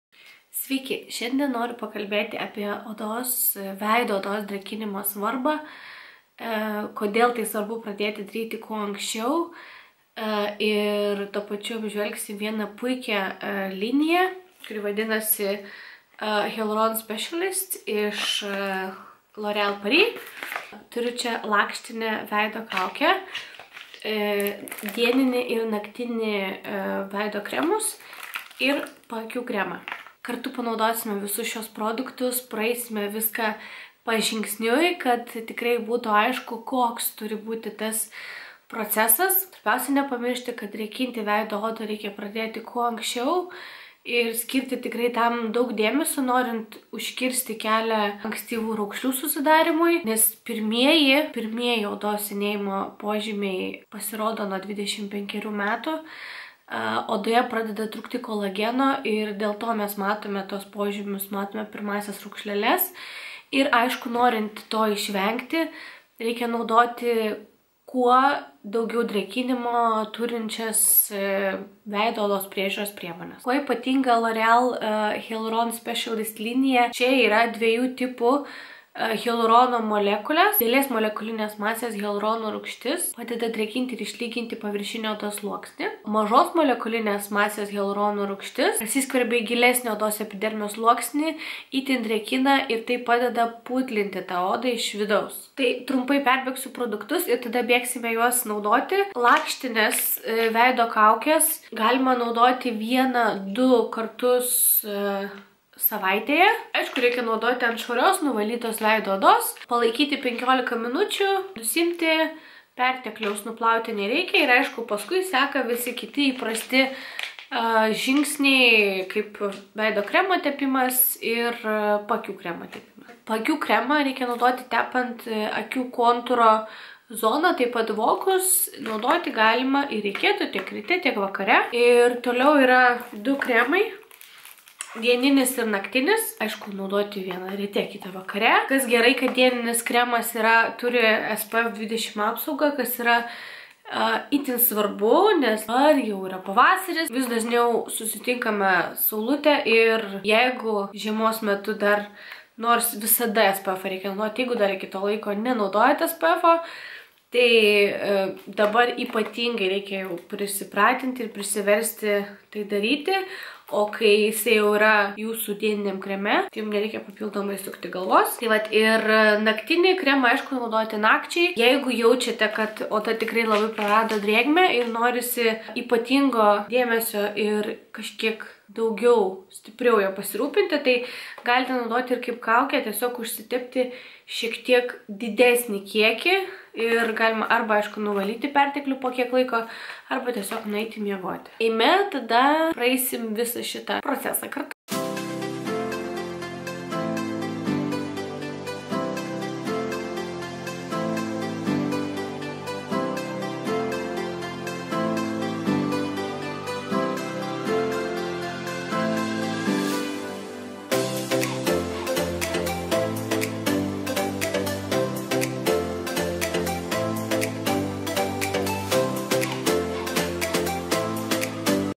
L’Oreal Hyaluron Specialist veido priežiūros linijos apžvalga
Ar žinojai, kad pirmieji odos senėjimo požymiai pasirodo jau nuo 25 metų, kai odoje pradeda trūkti kolageno? Vis dėlto, puoselėjant ir rūpinantis oda intensyviai drėkinančiomis veido odos priežiūros priemonėmis, ją galima išlaikyti sveiką ir gražią gana ilgą laiką. Sandra supažindins su L’Oreal Hyaluron Specialist veido priežiūros linija, kurią sudaro dieninis ir naktinis veido kremai, veido kaukė ir paakių kremas. Žiūrėk jau dabar ir nepamiršk pasirūpinti savo veido oda iš anksto! 😊👆